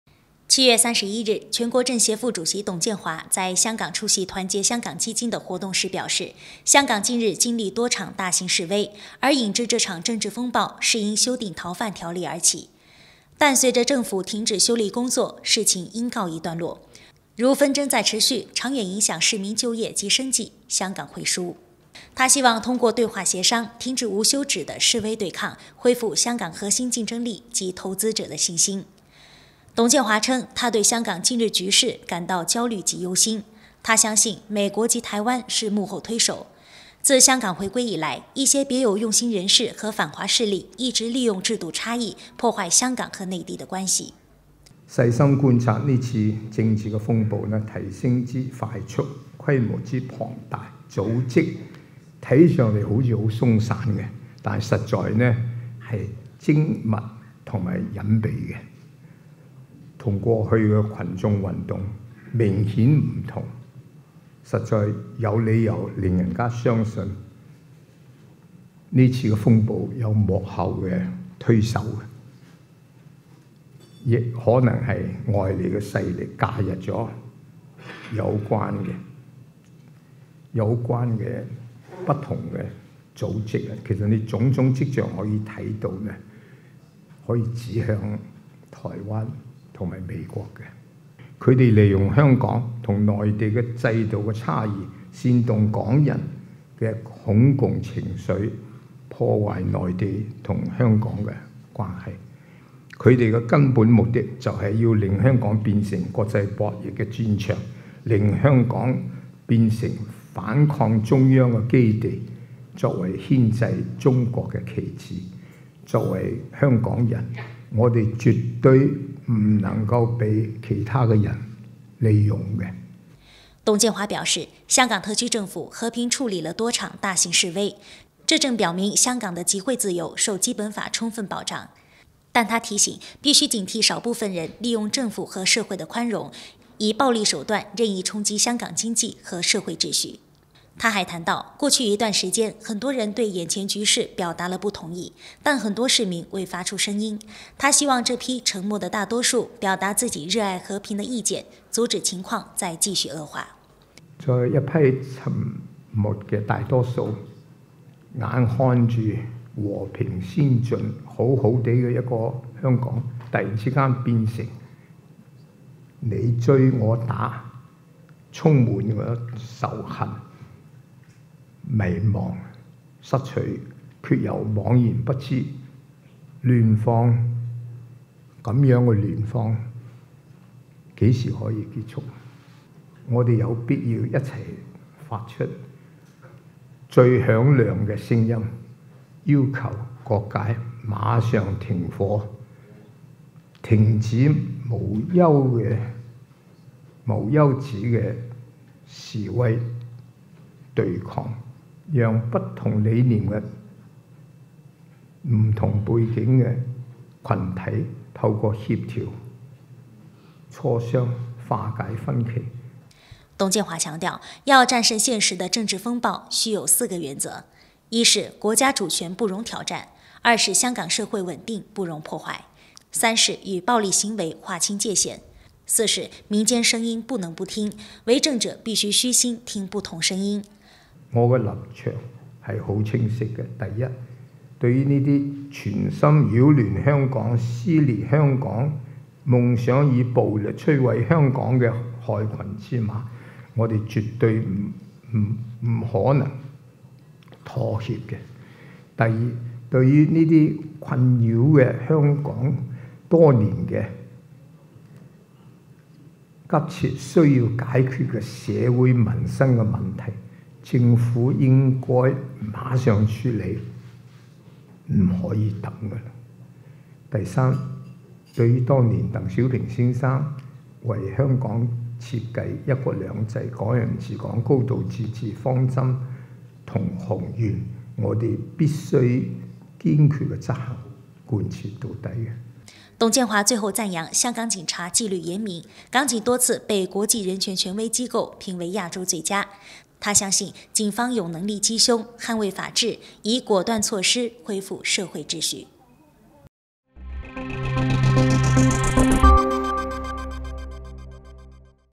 董建華：香港政治風暴有美國台灣勢力介入 對害群之馬絕不會妥協
7月31日，全國政協副主席董建華在香港出席團結香港基金的活動時表示，香港近日經歷多場大型示威，而引致這場政治風暴是因修訂《逃犯條例》而起，但隨著政府停止修例工作，事情應告一段落，如（紛爭）再持續，長遠影響市民就業及生計，香港會輸。他希望通過對話協商，停止無休止的示威對抗，恢復香港核心競爭力及投資者的信心。董建華稱他對香港近日局勢感到焦慮及憂心，他相信美國及台灣是幕後推手，自香港回歸以來，一些別有用心人士和反華勢力一直利用制度差異，破壞香港和內地的關係。董建華強調，要戰勝現時的政治風暴需有四個原則，一是國家主權不容挑戰；二是香港社會穩定不容破壞；三是與暴力行為劃清界線；四是民間聲音不能不聽，為政者必須虛心聽不同聲音。